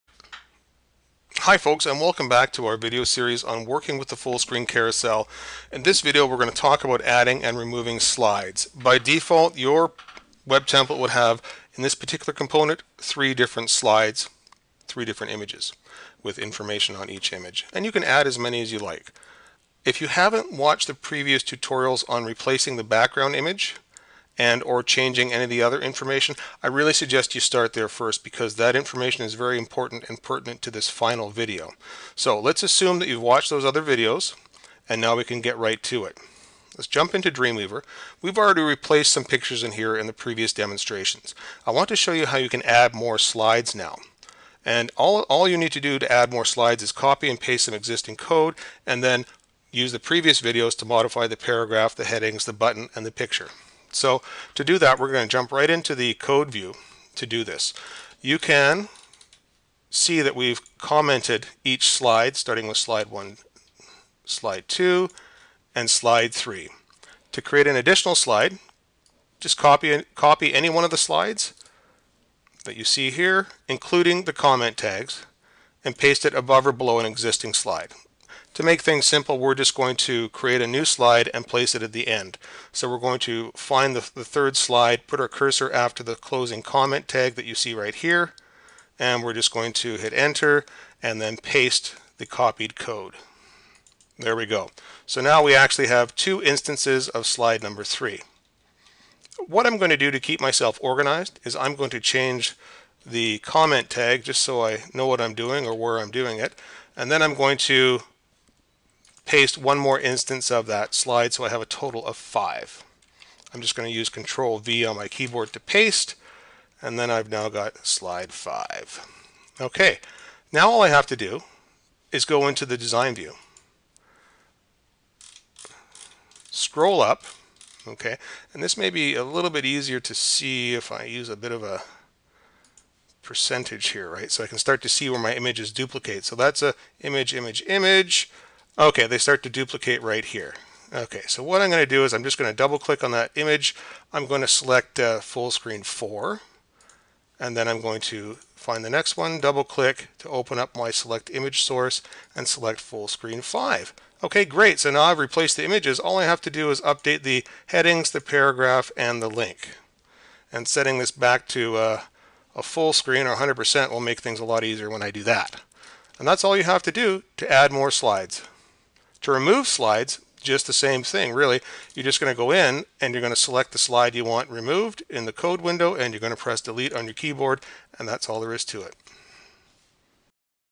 Add and remove slides from the Fullscreen Carousel Dreamweaver
Learn how to Add and remove slides from the Fullscreen Carousel using Dreamweaver.